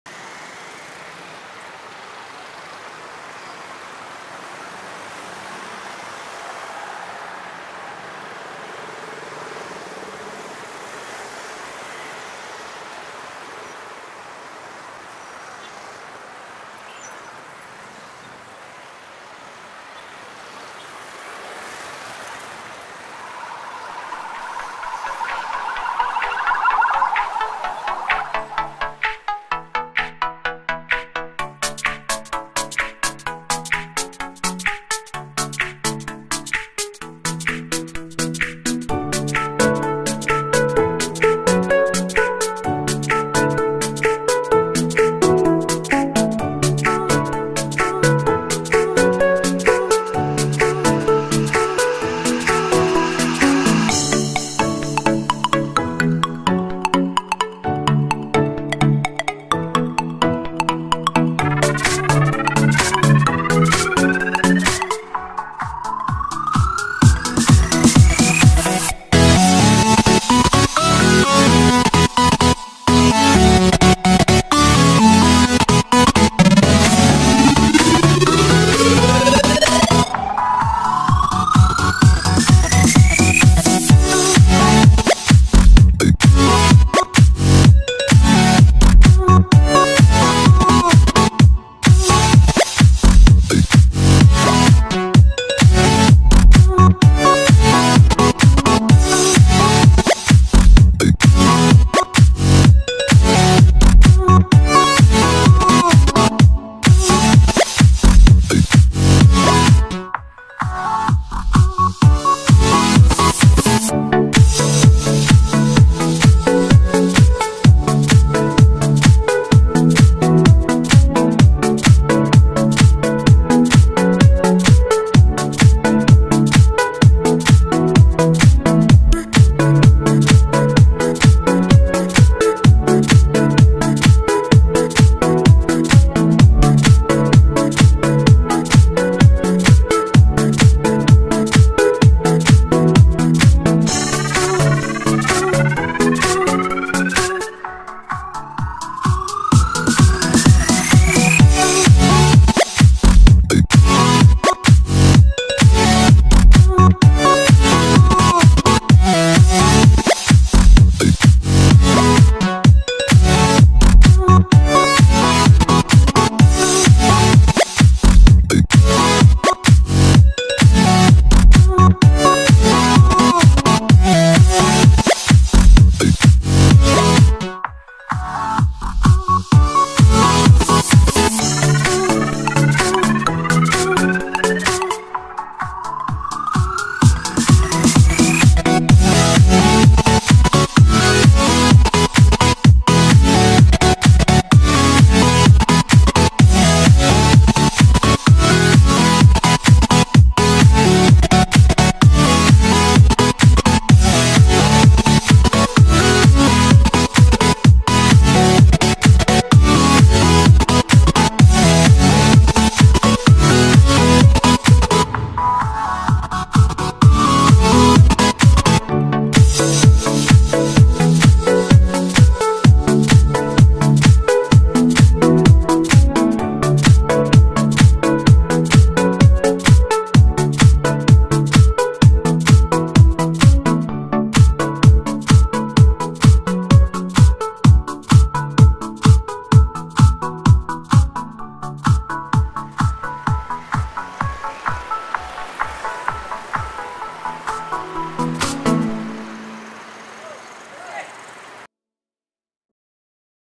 Kathmandu / Durbar Marg Timelapse in 360 VR
Kathmandu, Kingsway (Durbar Marg ) timelapse in 360 Virtual Reality (VR) mode.